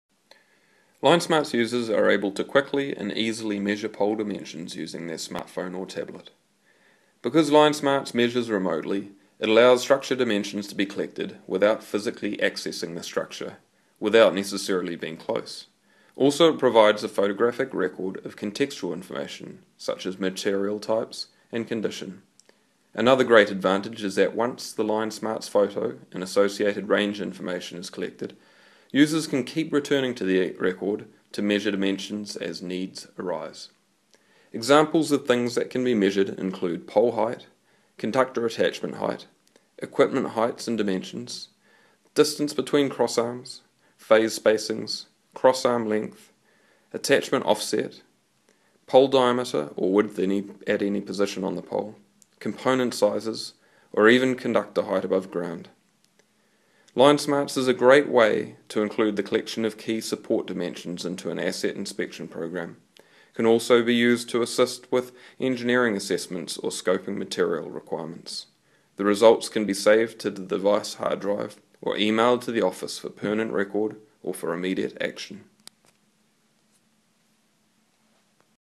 Dimensional measurement - Introduction
This videos provides an introduction to LineSmarts' dimension measurement functionality.

LineSmarts is an app that turns an android smartphone or tablet into a sophisticated analysis and measurement tool for overhead power lines and structures. LineSmarts uses a novel method for remotely capturing a variety of measurements. You simply take a photo of the span or structure that you want to measure, input the distance to the structure or structures, and then use the tools in LineSmarts to make measurements from the photo. Current features include:
*Measurement of wire tension
*Measurement of wire catenary constants
*Measurement of wire sag
*Measurement of span length
*Calculation of conductor tension under design load cases
*Measurement of pole lean
*Measurement of pole horizontal and vertical dimensions